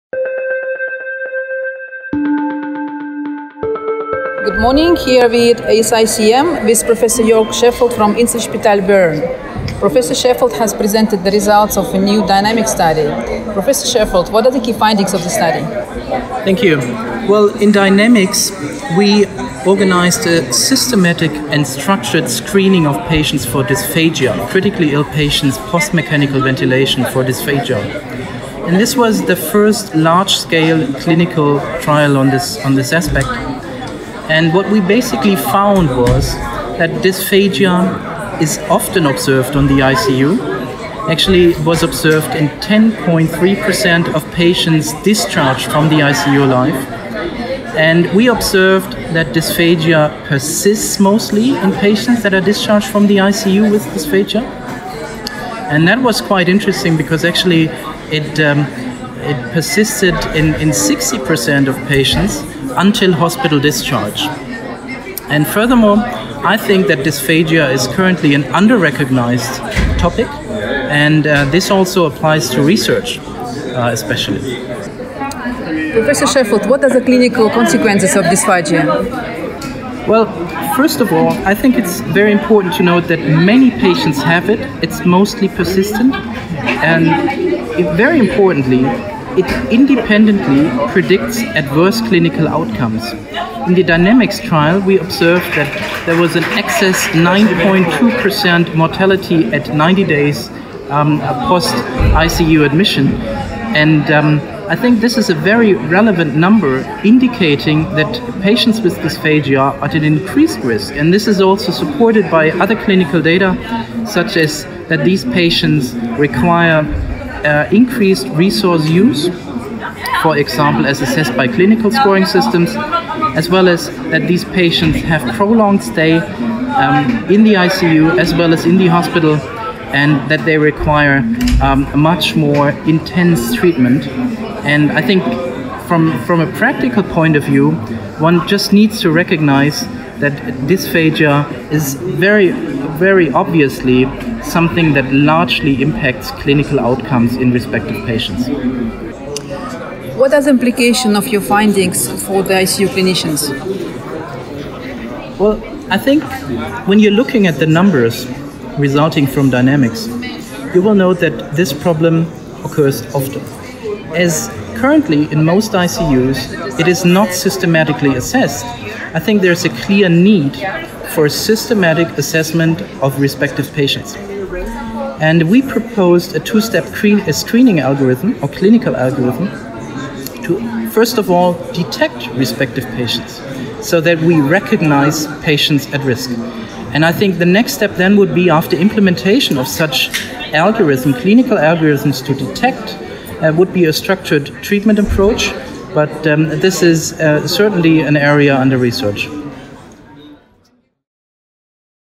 ESICM Paris 2018 - Impacting Dysphagia Outcomes - Dynamics Trial - Prof. Joerg C. Schefold
Interview with Professor Joerg C. Schefold of Inselspital, Universitätsspital Bern, by Natalia Muehlemann, Nestlé Health Science

From ESICM Paris 23rd October 2018